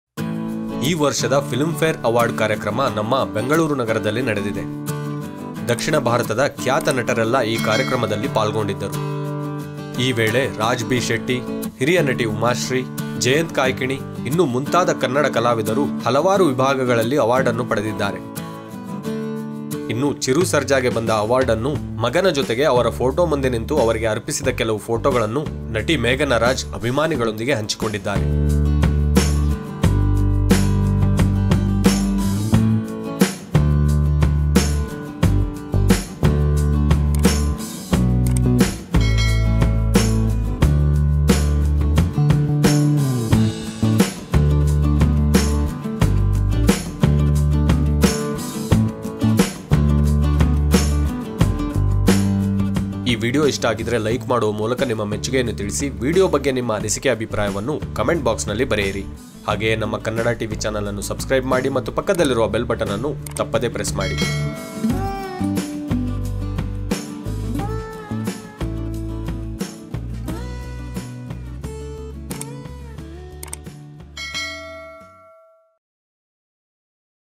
ಅಪ್ಪನ ಅವಾರ್ಡ್ ಕಸಿದು ಕೊಂಡ ರಾಯನ್ ರಾಜ್ ಸರ್ಜಾ| Meghana Raj baby with Filmfare Awards 2022 | Kannada Tv
ನಟಿ ಮೇಘನಾ ರಾಜ್, ಪತಿಯ ಅವಾರ್ಡ್ ಮಗನ ಜೊತೆಗೆ ಫೋಟೋ ತೆಗೆಸಿಕೊಂಡ ಸುಂದರ ಕ್ಷಣಗಳು  | Meghana raj film fare award. Meghan raj son with chiru sarja photo. Raayan raj sarja with chiru sarja photo.